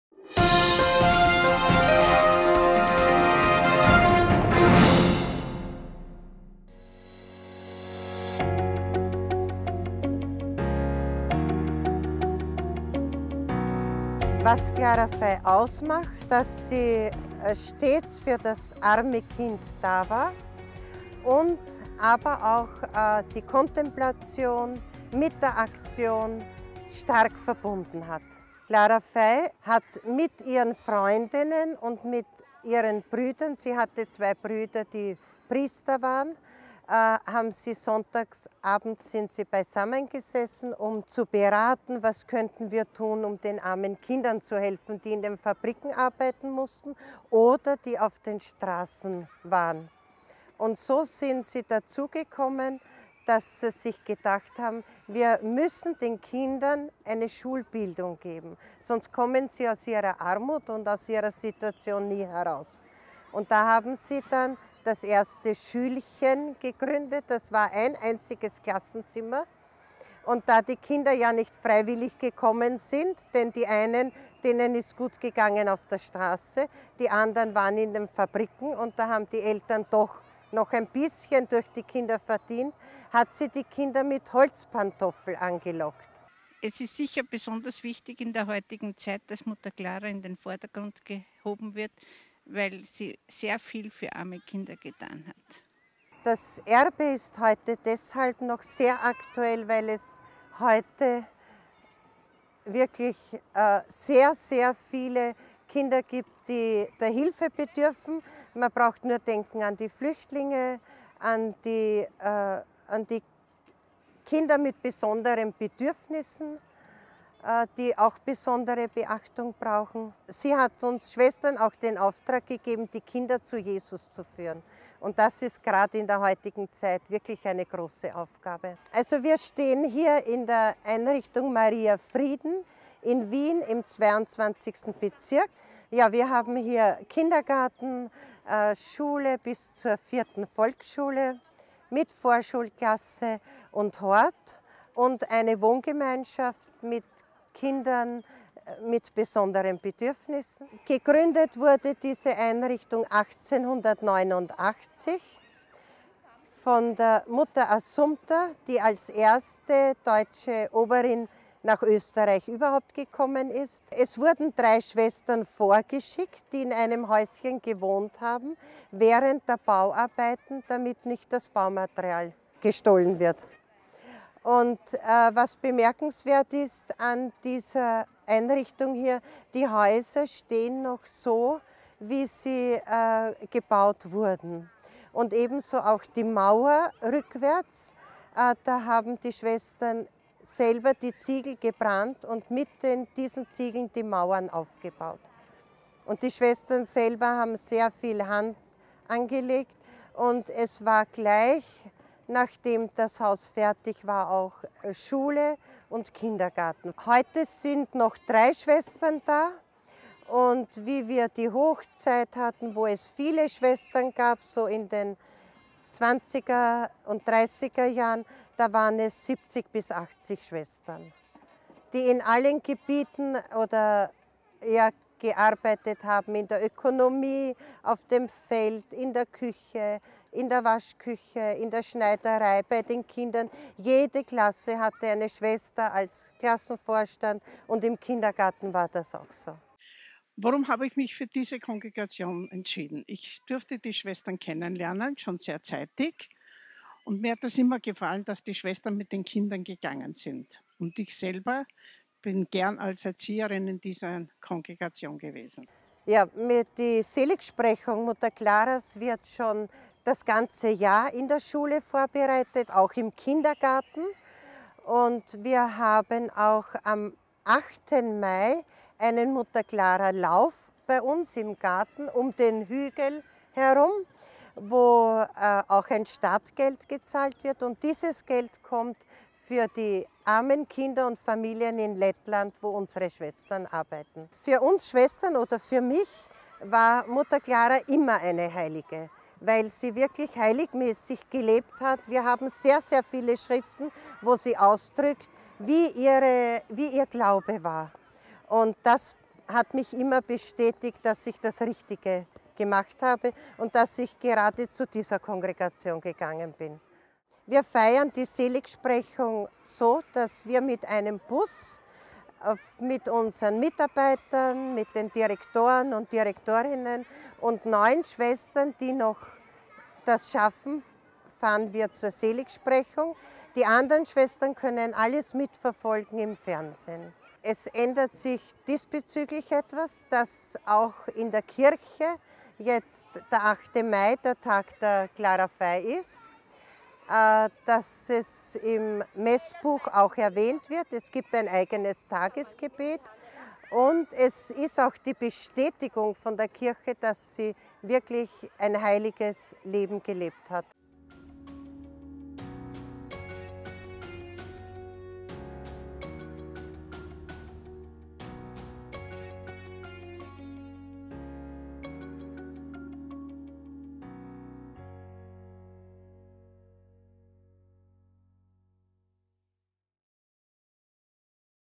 Das Erbe der Clara Fey- Kongregation der Schwestern vom armen Kinde Jesus. Schule Maria Frieden
Im Mai 2017 hat Papst Franziskus das Dekret zur Seligsprechung der Aachener Ordensgründerin Clara Fey autorisiert. „Mit dieser Entscheidung wird die Gründerin des Ordens der Schwestern vom armen Kinde Jesus für ihren Glauben, ihre Spiritualität und ihr Werk der tätigen Nächstenliebe gewürdigt", sagt der Aachener Bischof, Dr. Helmut Dieser.

Weitere Informationen zur Ordensgründerin Clara Fey und zur Kongregation der Schwestern vom armen Kinde Jesus

Schule:
Gelebte Schulpartnerschaft ist uns in der privaten Volksschule Maria Frieden ein besonderes Anliegen. Wissensvermittlung erfolgt mit dem Ziel, das Interesse der Schülerinnen Schüler zu wecken, Begabungen zu fördern und Lernschwächen durch besondere Förderung zu mildern.

Bleiben wir verbunden!